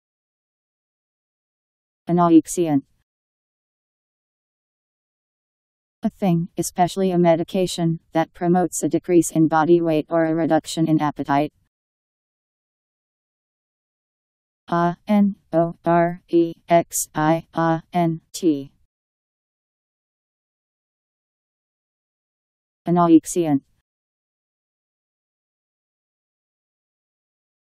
Anorexiant Meaning
Video shows what anorexiant means. A thing, especially a medication, that promotes a decrease in body weight or a reduction in appetite..  Anorexiant Meaning. How to pronounce, definition audio dictionary. How to say anorexiant. Powered by MaryTTS, Wiktionary